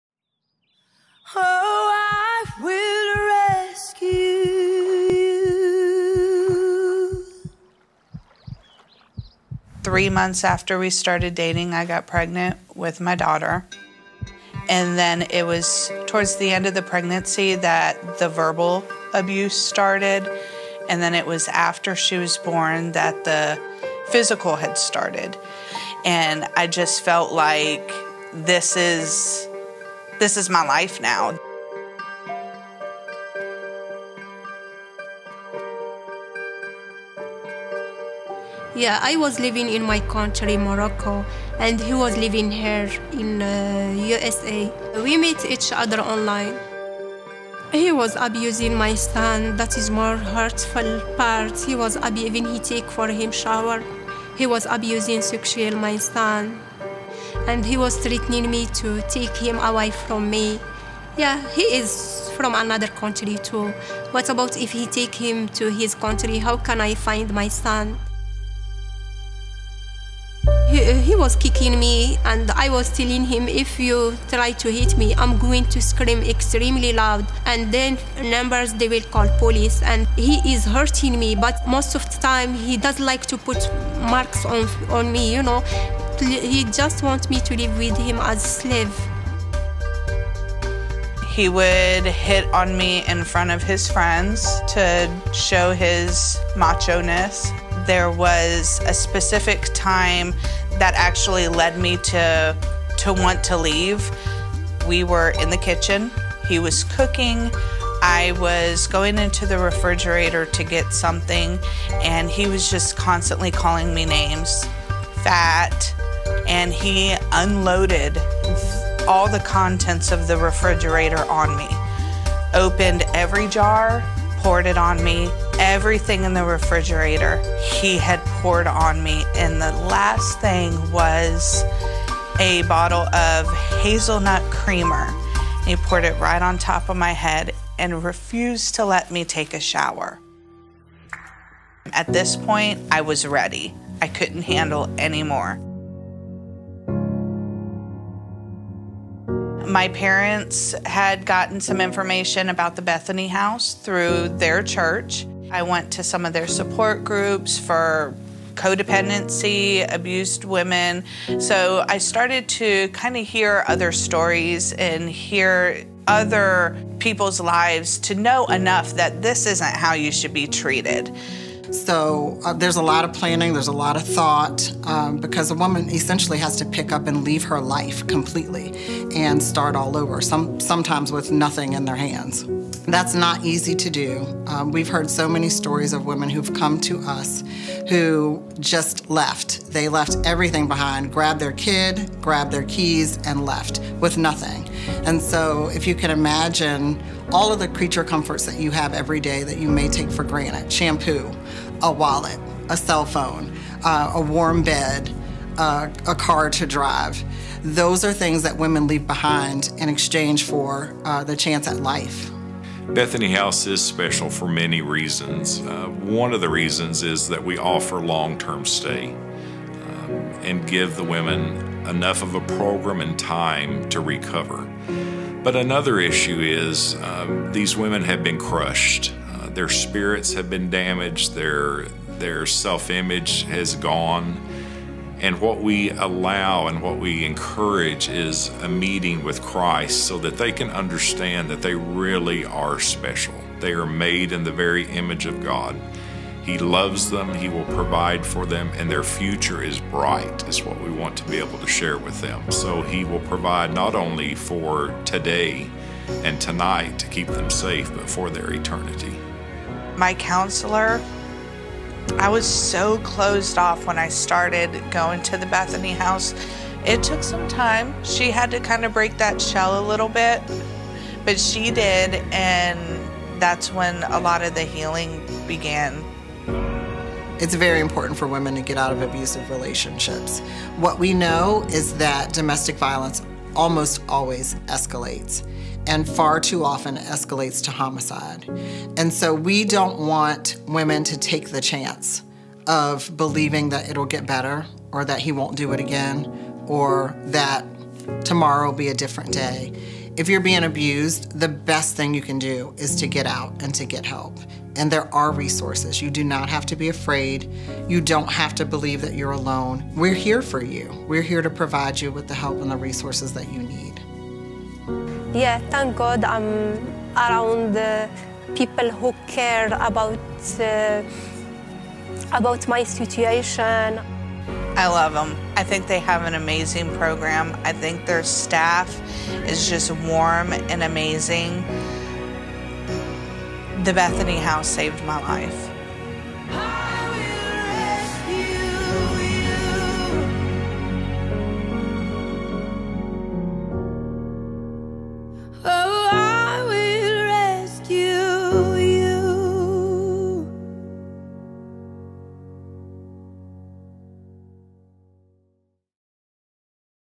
Bethany House Saved by Life
Survivor stories and what Bethany House does to help those who have been abused.  Directed by Schwartzy.